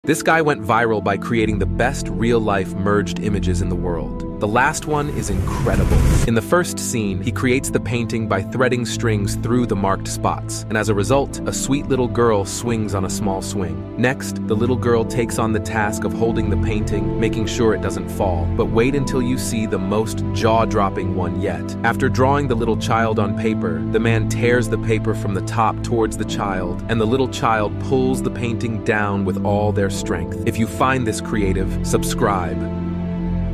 Creative Real-life Art Project 🤩
Be sure to check out @andrewscott_art on Instagram

In the first scene, he creates the painting by threading strings through the marked spots, and as a result, a sweet little girl swings on a small swing.
Next, the little girl takes on the task of holding the painting, making sure it doesn’t fall. But wait until you see the most jaw-dropping one yet.
After drawing the little child on paper, the man tears the paper from the top towards the child, and the little child pulls the painting down with all their strength. Do you think this is creative?